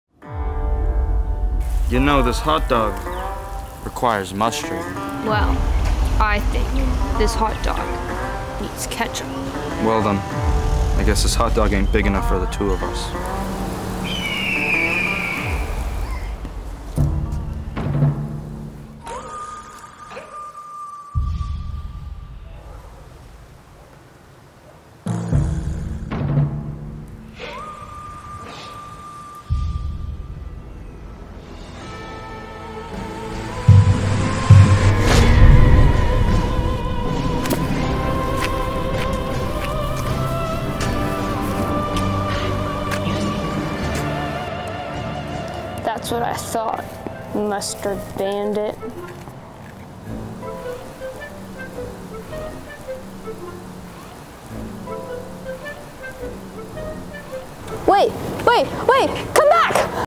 The Hotdog Duel - Vertical Short
Ketchup VS Mustard who will come out on top